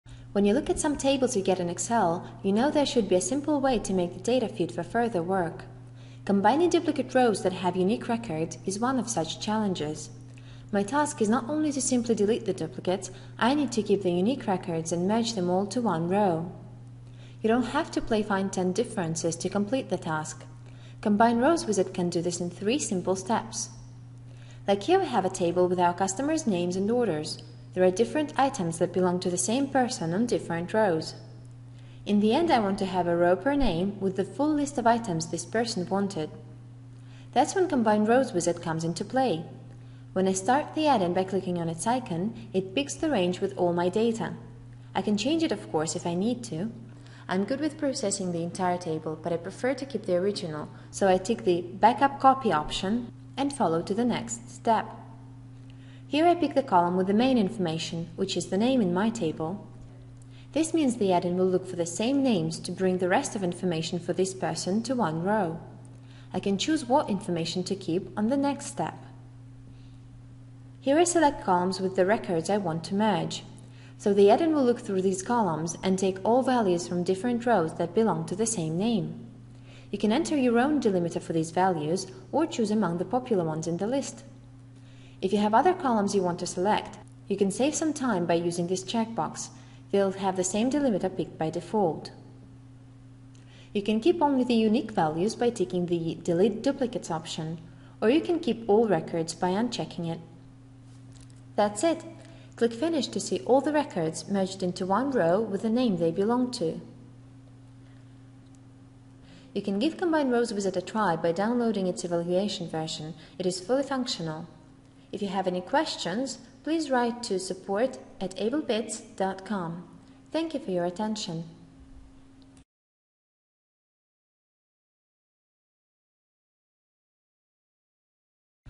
Combine Duplicate Rows in Excel in 2 minutes.
See how to quickly merge duplicate rows keeping unique data only. Download the Combine Rows Wizard for free